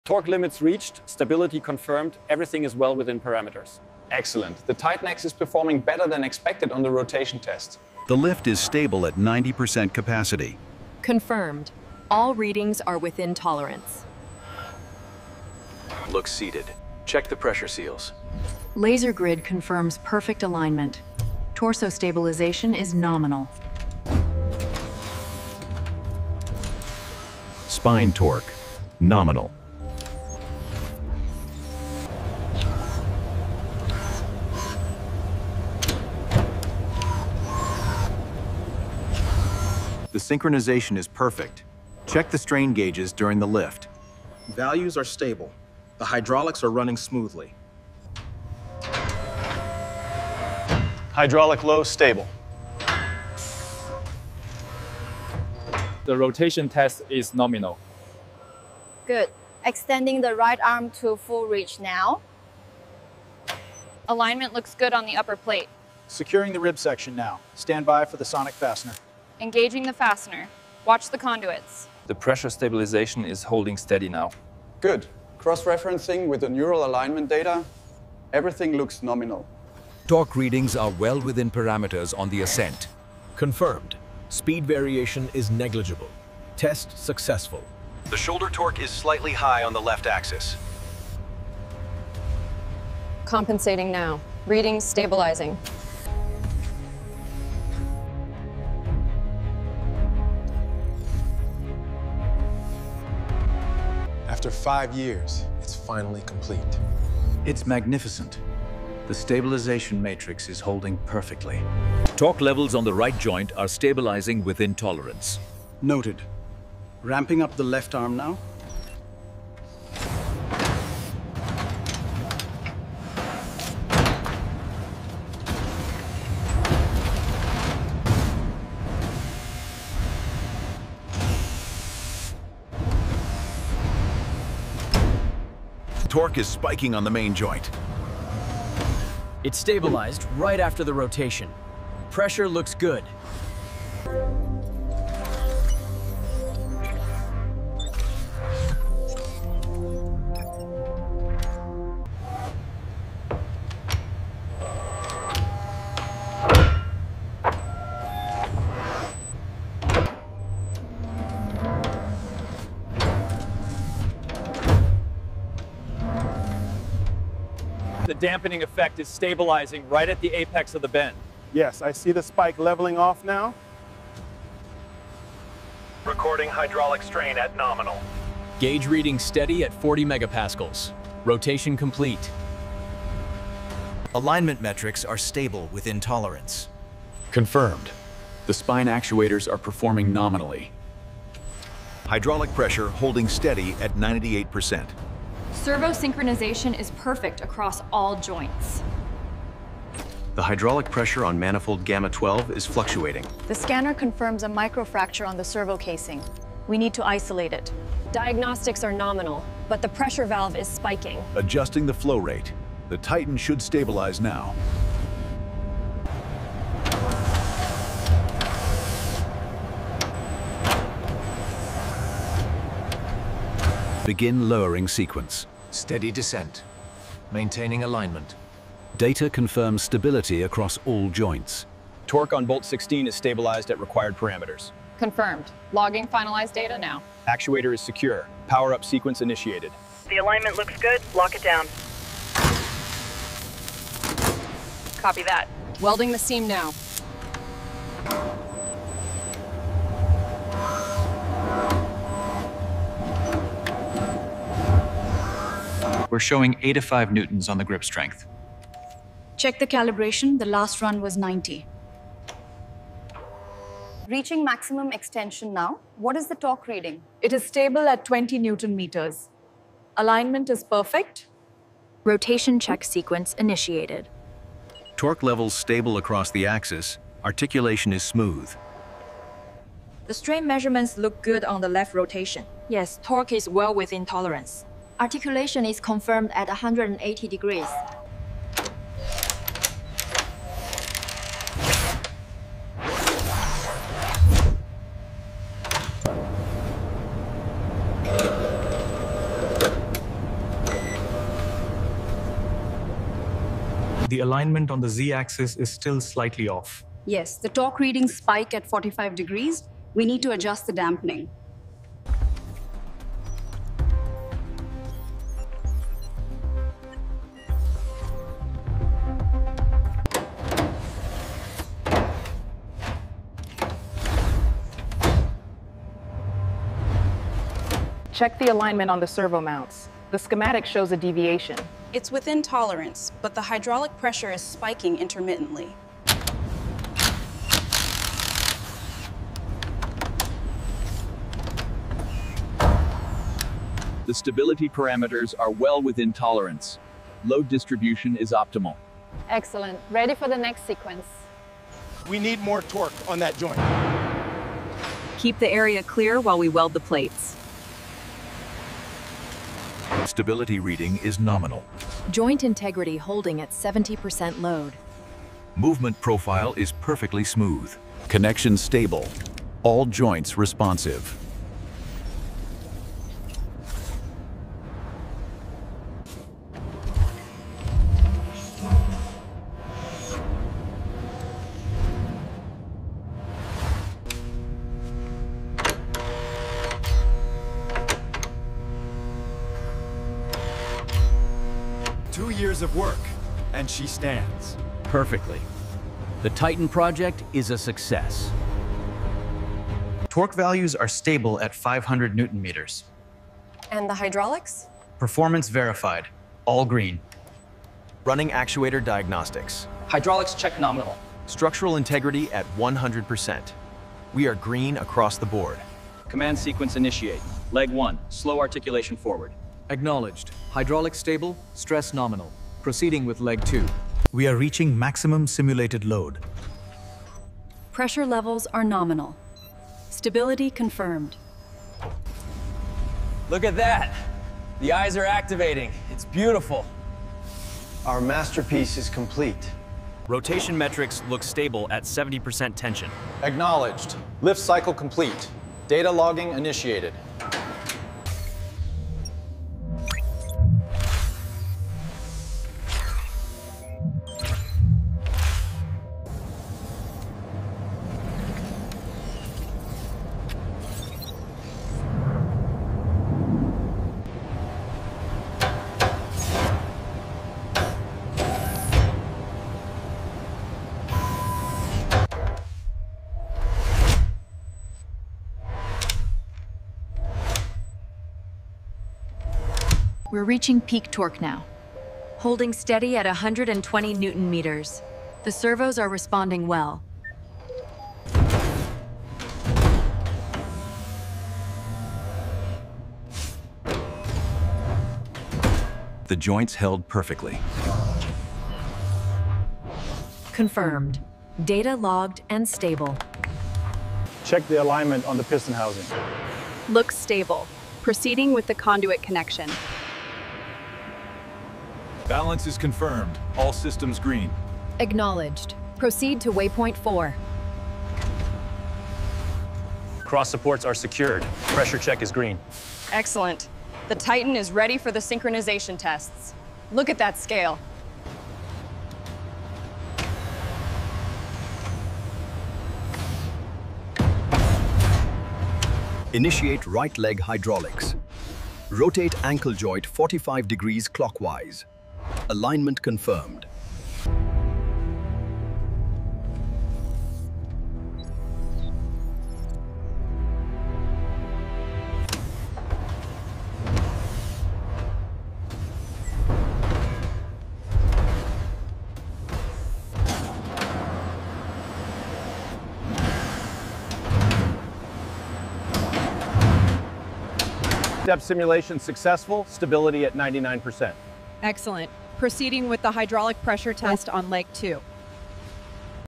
This Is the Robot That Humans Built on Earth
This video shows a 60-year-old inventor slowly building a human-like robot from the ground up. Step by step, he creates every part with his own hands — assembling the body, connecting the systems, and shaping her into a perfect robot girl. Watch the full process of how one man’s knowledge, patience, and skill bring a life like robot to life.